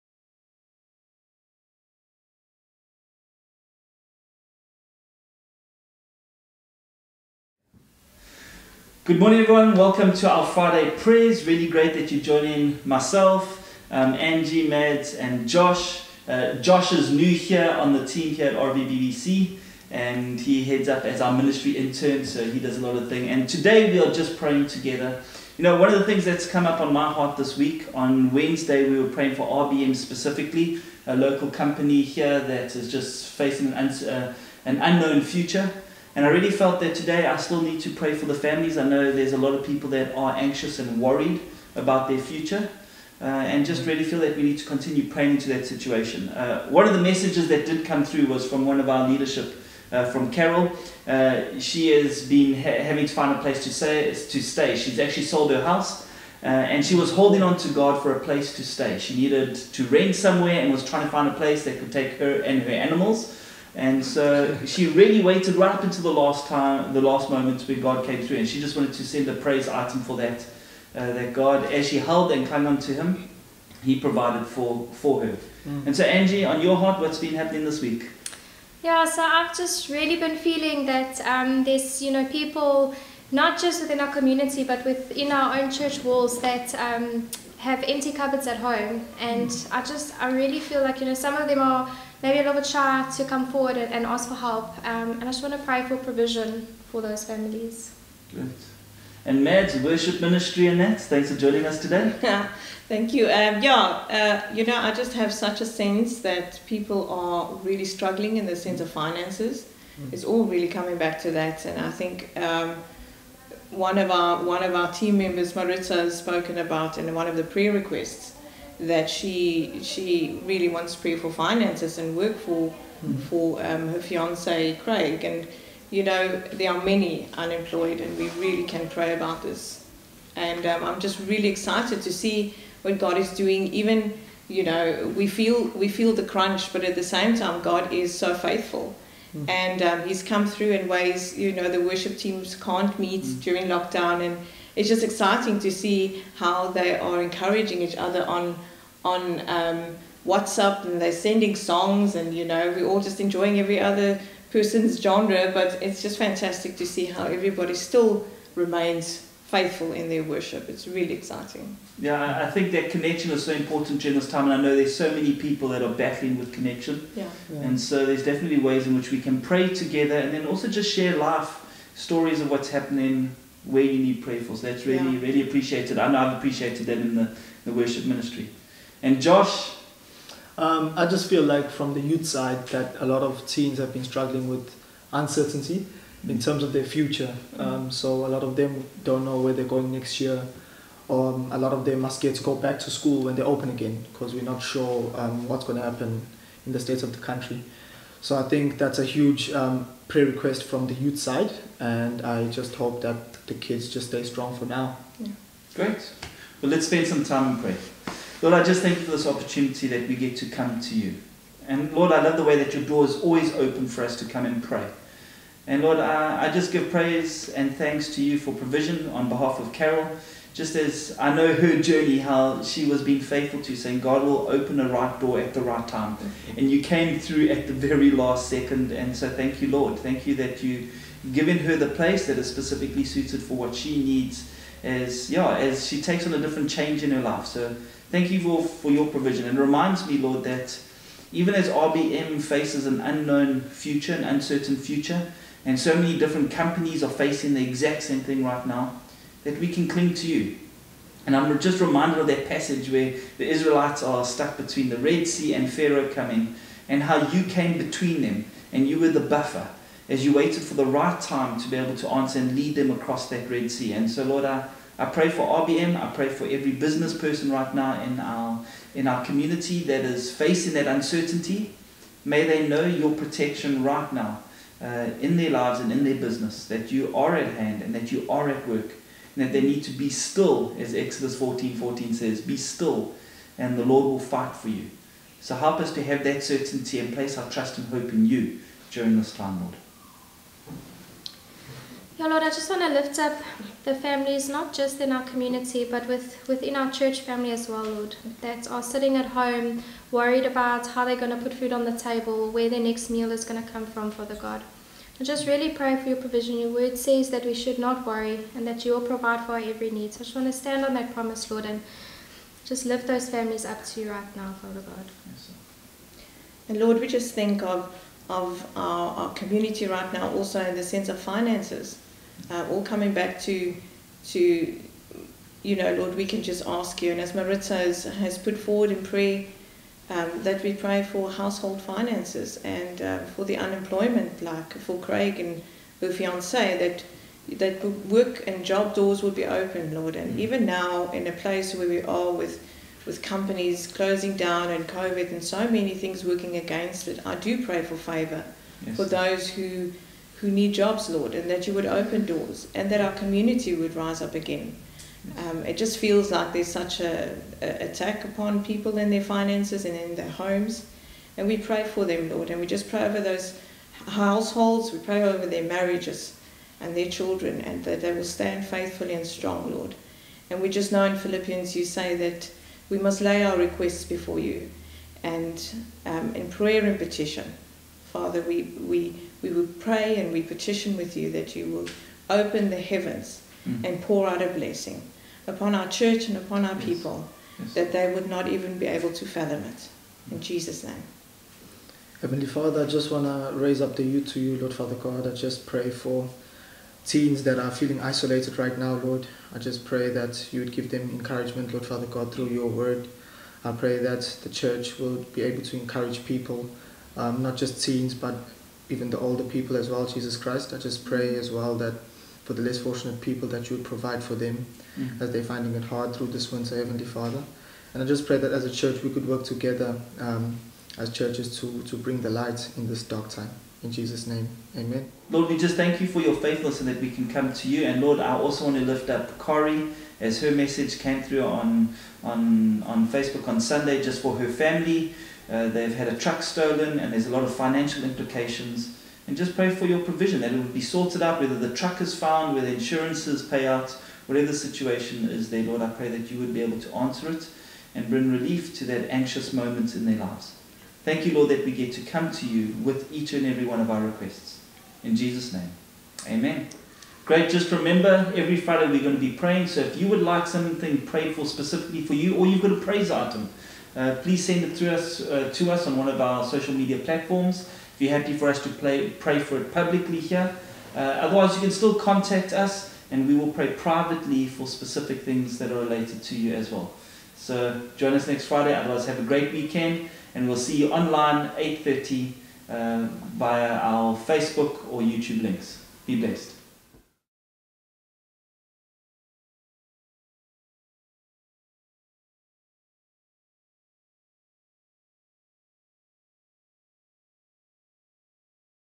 Prayer Fridays 09-07-21
Morning! On Fridays we will be praying together to end off the week.

This weeks prayer focus:
• Local mining company RBM future in Zululand
• Homes without food
• Young people feeling anxious about their future

If you are new- keep connected for more message like this and other ways to grow:

If you haven’t already, join one of our online video channels

Why not invite others to join us!

Praying you have a blessed weekend- See you OnLine on Sunday @08:30